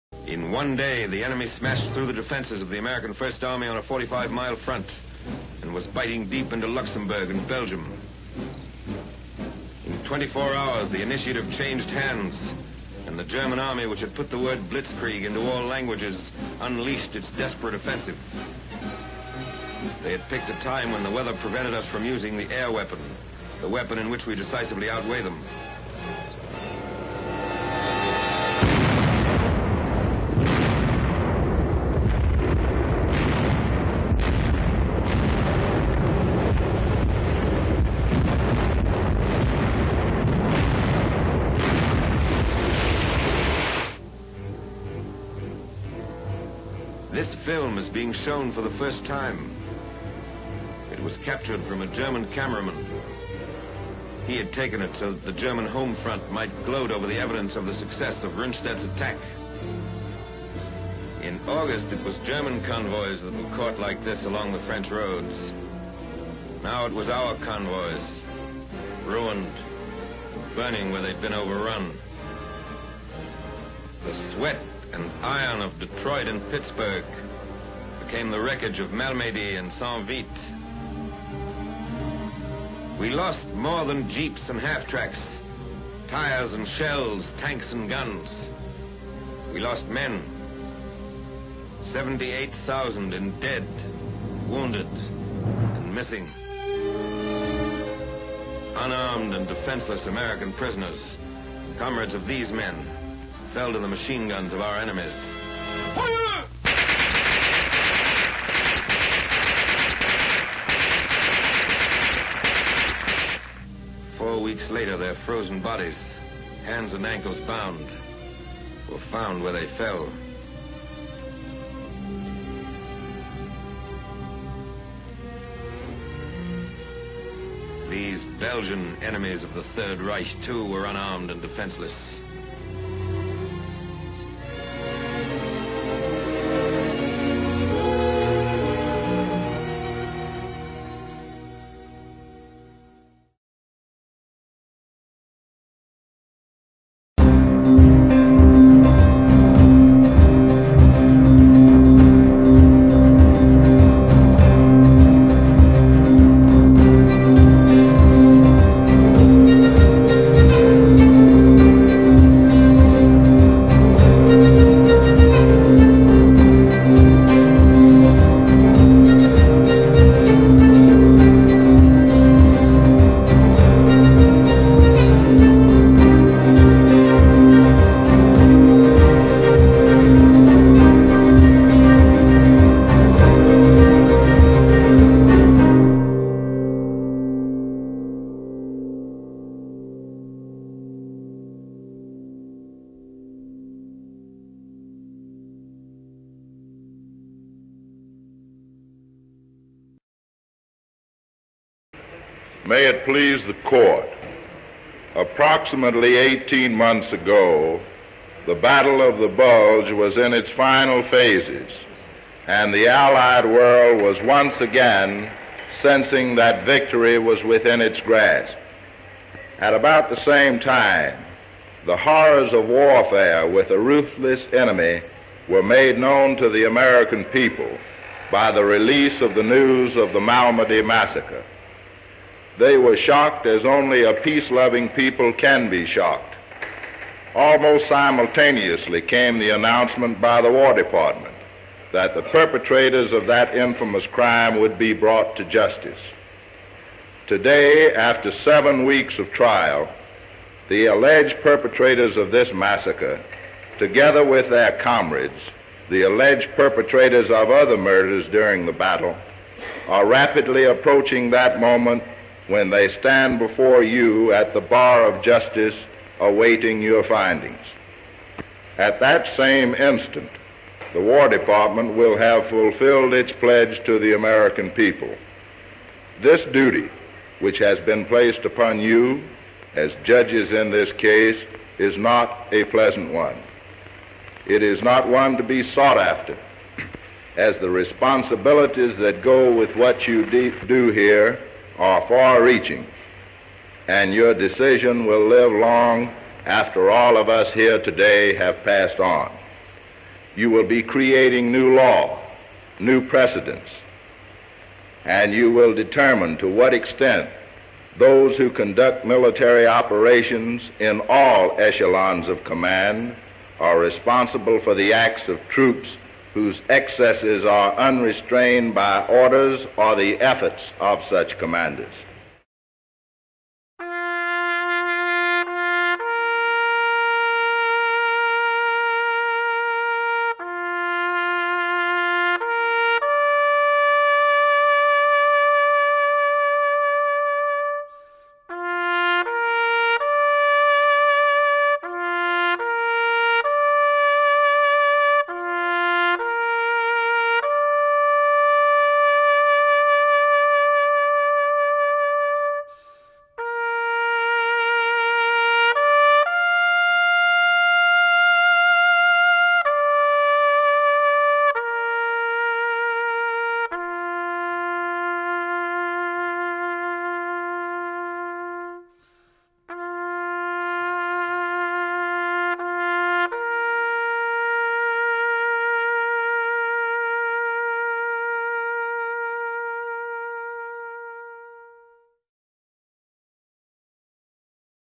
The Malmedy Massacre (Note: Graphic images and subject matter)
The Malmedy massacre was a war crime committed by members of Kampfgruppe Peiper (part of the 1st SS Panzer Division), a German combat unit, at Baugnez crossroads near Malmedy, Belgium, on December 17, 1944, during the Battle of the Bulge. According to numerous eyewitness accounts, 84 American prisoners of war were massacred by their German captors: the prisoners were assembled in a field and shot with machine guns.

The term Malmedy massacre also applies generally to the series of massacres committed by the same unit on the same day and following days, which were the subject of the Malmedy massacre trial, part of the Dachau Trials of 1946. The trials were the focus of some controversy.